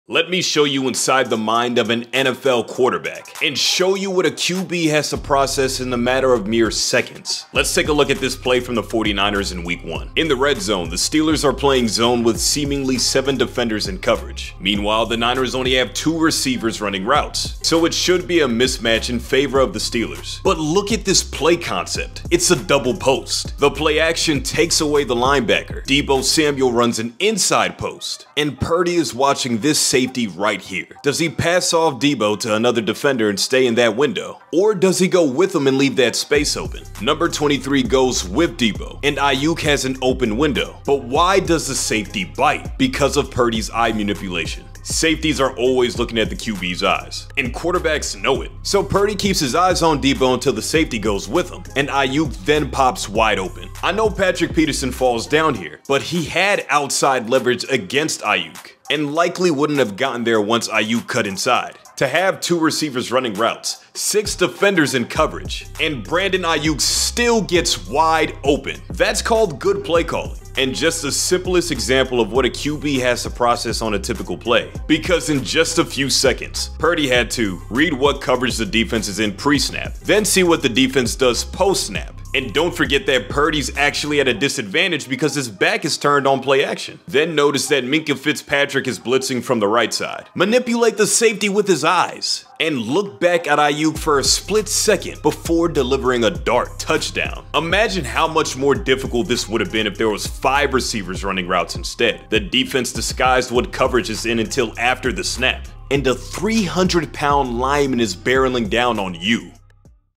Are You SMARTER Than An NFL QB?
Ever wonder everything a QB has to process in a play? Tom Brady and Patrick Mahomes are 2 of the best to ever do it, but let’s look at a regular play from the San Francisco 49ers against the Pittsburgh Steelers in Week 1. And see just how much QB Brock Purdy has to process in mere seconds.